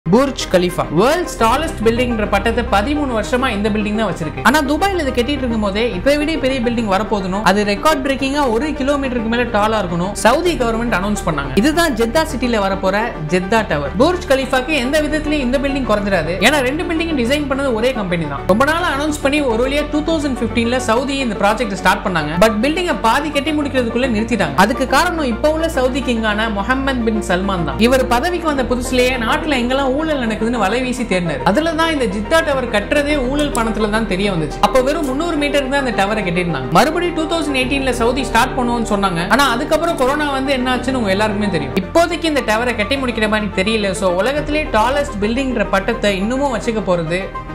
Burj Khalifa-விட பெரிய Tower in Saudi Arabia! 🇸🇦
The Burj Khalifa, known as the Burj Dubai prior to its inauguration in 2010, is a skyscraper in Dubai, United Arab Emirates. It is known for being the world’s tallest building. With a total height of 829.8 m and a roof height of 830 m, the Burj Khalifa has been the tallest structure and building in the world since its topping out in 2009, supplanting Taipei 101, the previous holder of that status.
Jeddah Tower, previously known as Kingdom Tower, is a skyscraper construction project currently on hold. Located on the north side of Jeddah, Saudi Arabia, it is planned to be the first 1 km tall building, and the centerpiece and first phase of a development and tourist attraction known as Jeddah Economic City.
There was steady progress, but in January 2018, building owner JEC halted structural concrete work with the tower about one-third completed due to labor issues with a contractor following the 2017–2019 Saudi Arabian purge.